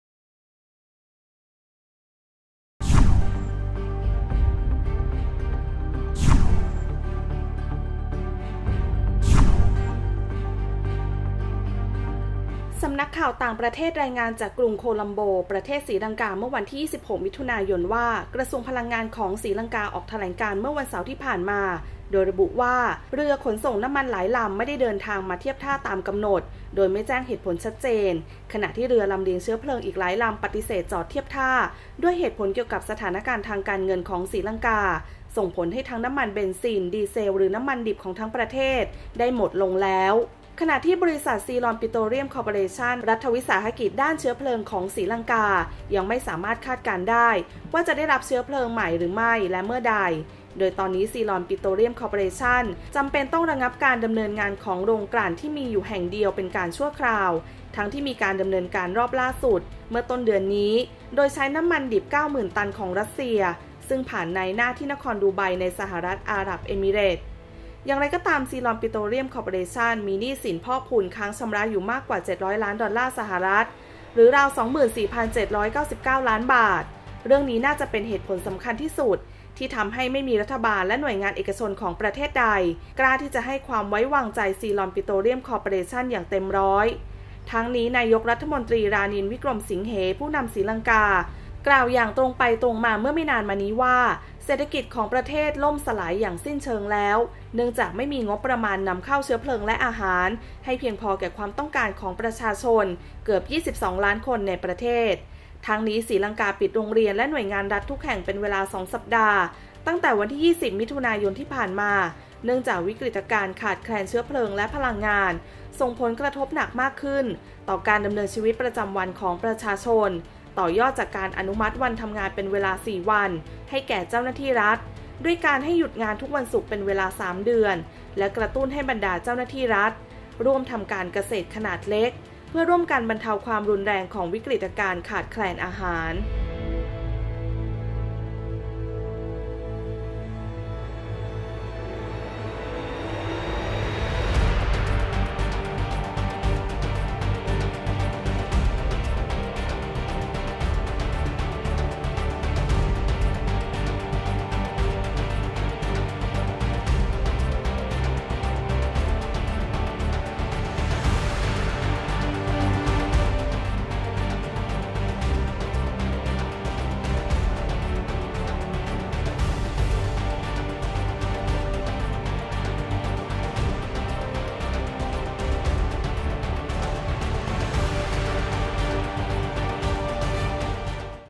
“ศรีลังกา” ล่มสลาย น้ำมันเกลี้ยงประเทศแล้ว | TopNewsทั่วไทย | TOP NEWS
เศรษฐกิจศรีลังกาพังทลาย หมดสิ้นแล้วทุกอย่าง นายกรัฐมนตรีรับเกินเยียวยา